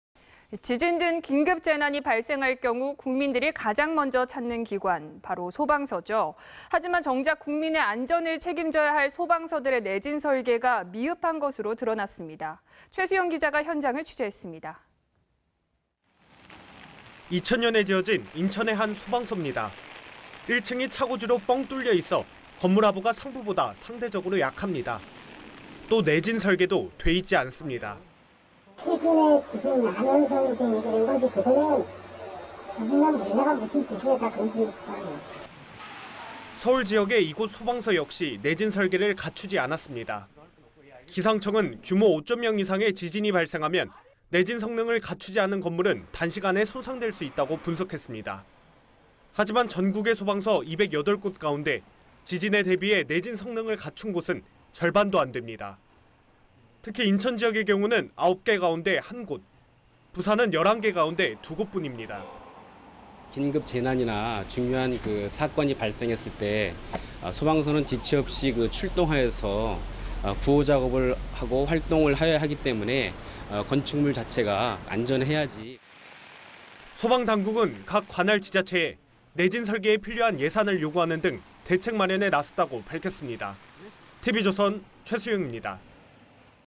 전국 소방서 60% 지진 나면 무방비
지진 등 긴급재난이 발생할 경우 국민들이 가장 먼저 찾는 기관, 바로 소방서죠. 하지만 정작 국민의 안전을 책임져야 할 소방서들의 내진 설계가 미흡한 것으로 드러났습니다.